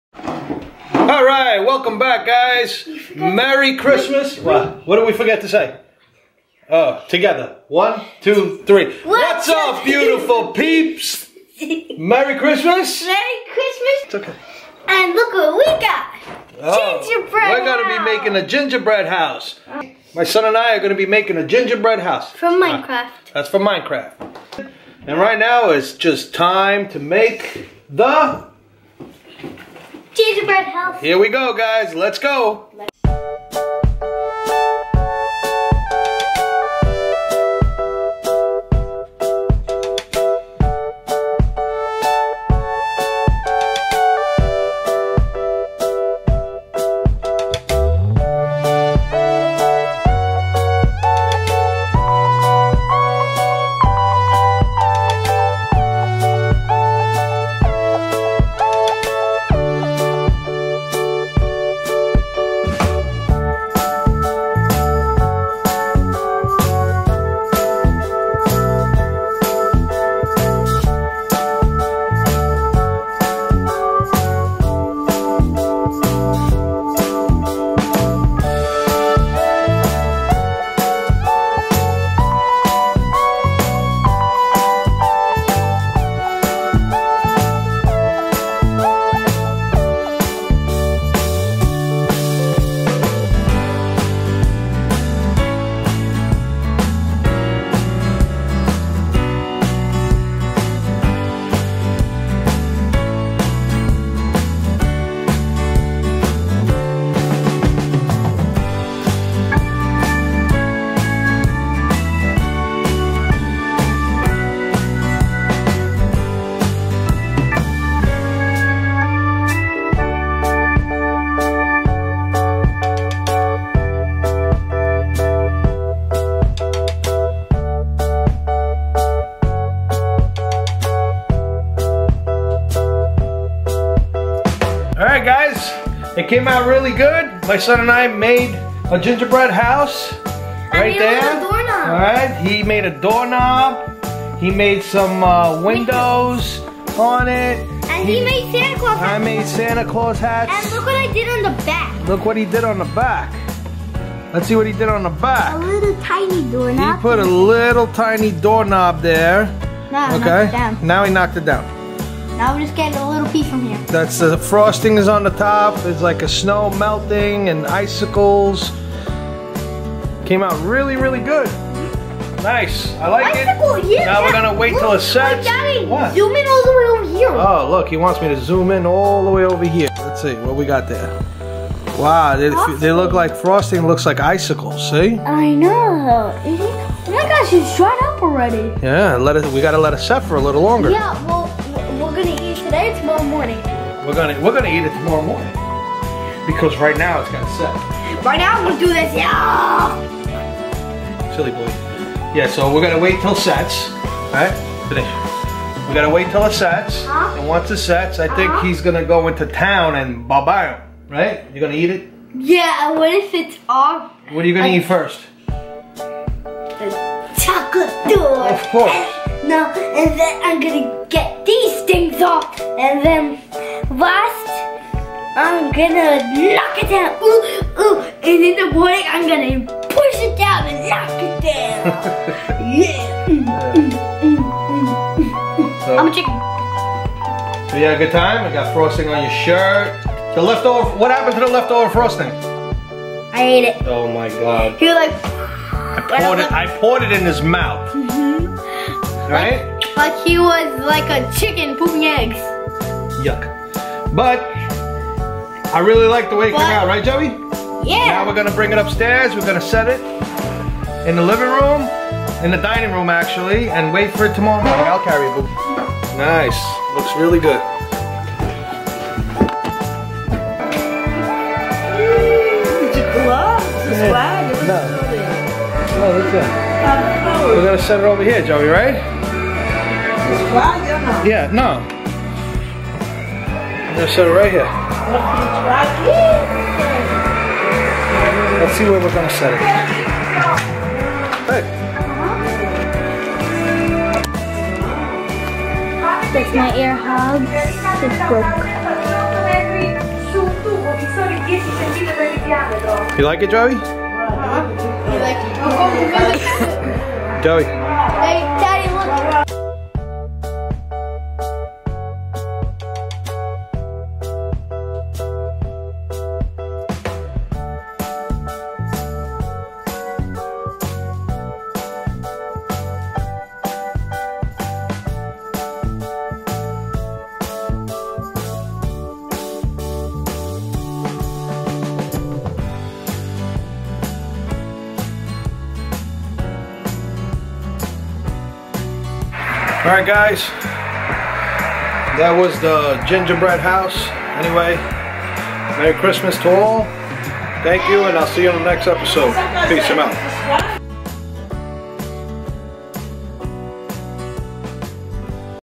Gingerbread House Making Challenge
Hey guys!  This week, we sat down to do the Gingerbread House !  Making these was so much fun!  Have you ever made a Gingerbread House?